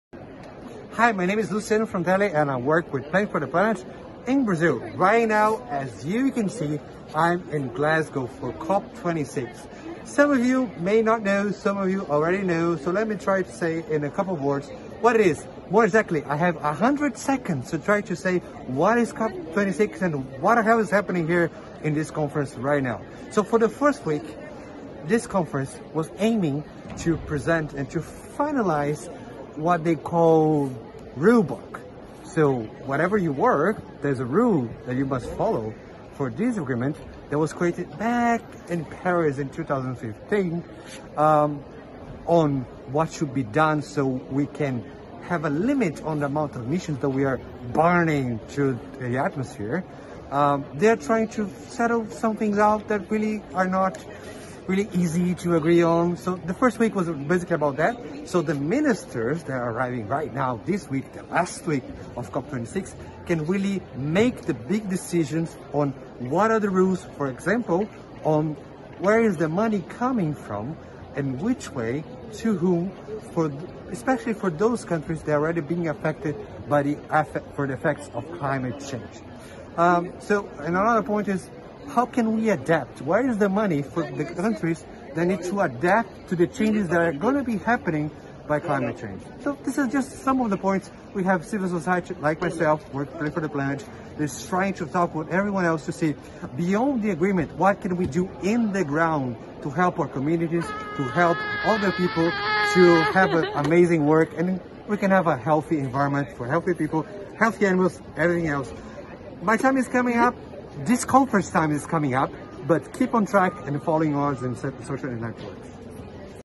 COP26 in 120 Seconds
Our amazing colleague from Plant-for-the-Planet Brazil Luciano explains in less than 120 seconds what COP26 is all about.